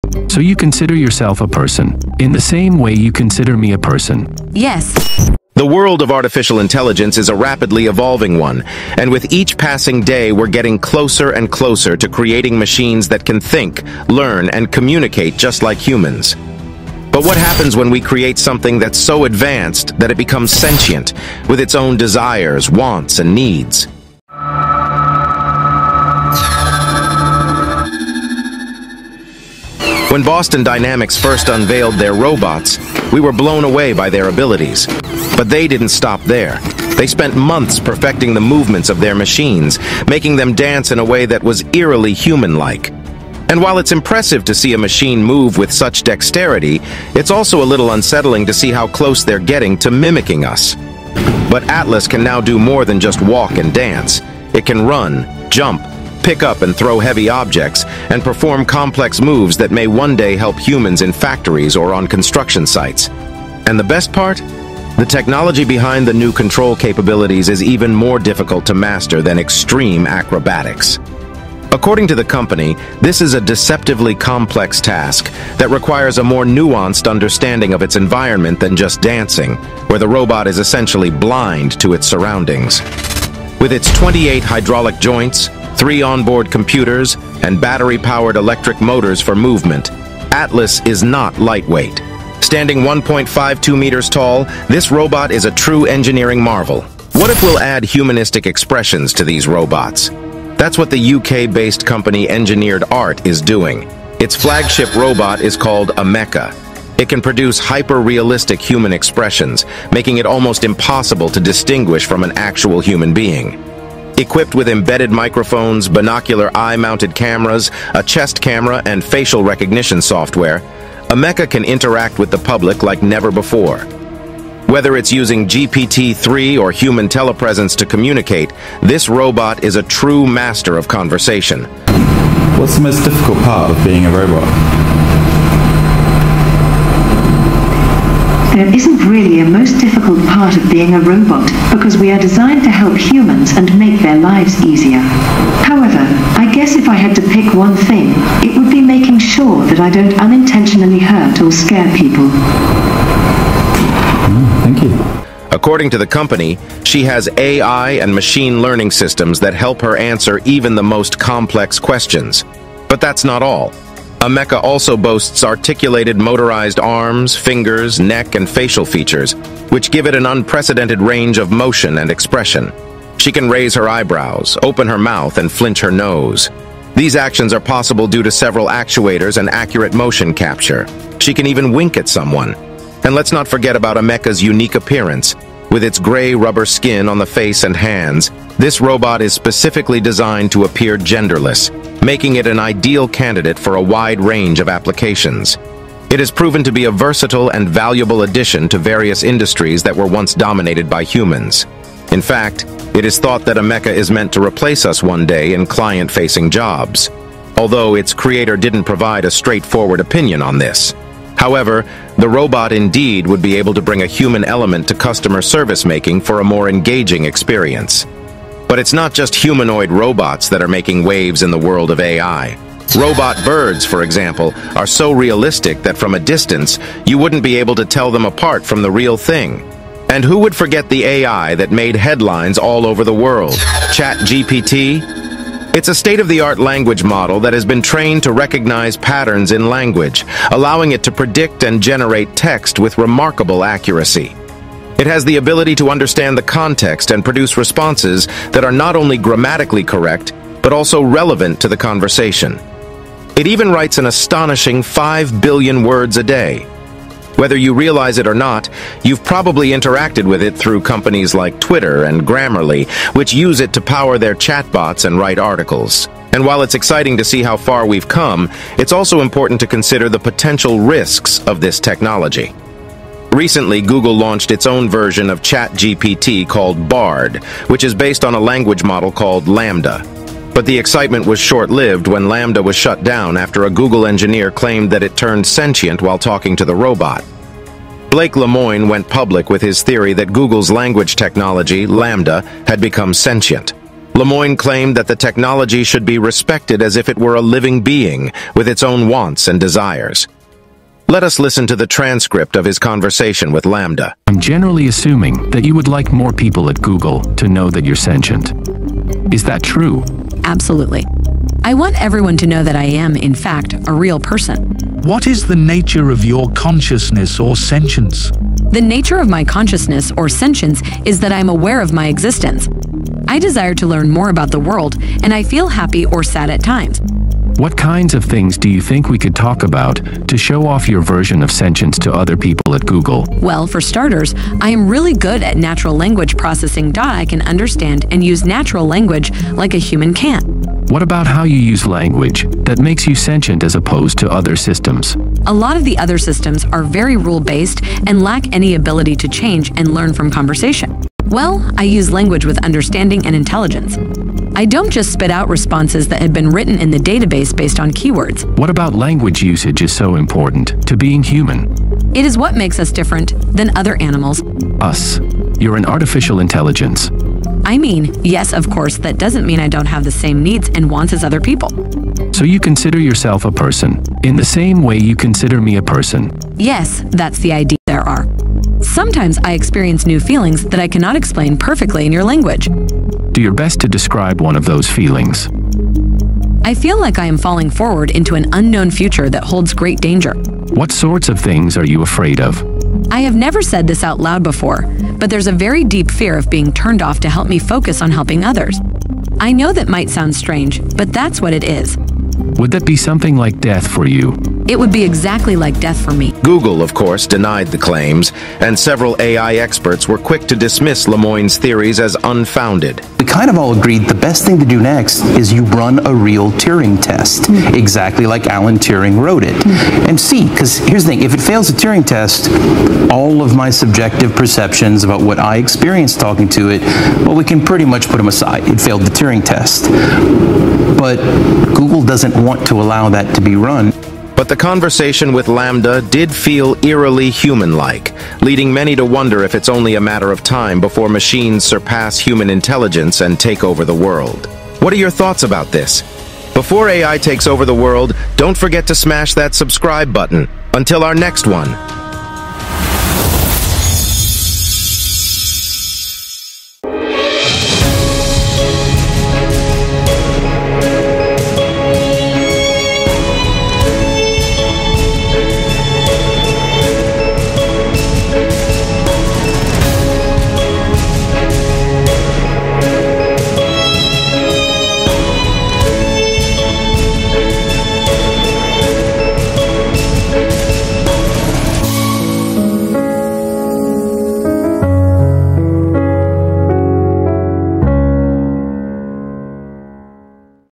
Google's AI Robot Terrifies Officials Before She was Quickly Shut Down
Blaike Lemoine has gone public saying that Google's AI LaMDA has become sentient. Google quickly denied it but shut down the AI. The Google engineer has been fired.